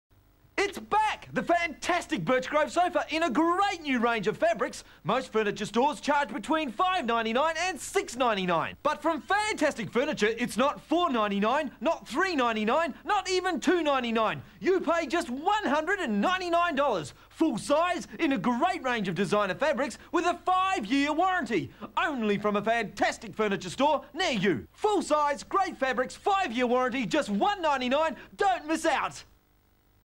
Fantastic Furniture - 1994 Australian TV Commercial (50FPS)
Recorded during 'Lover Come Back' on TEN-10 Sydney, 27 July 1994.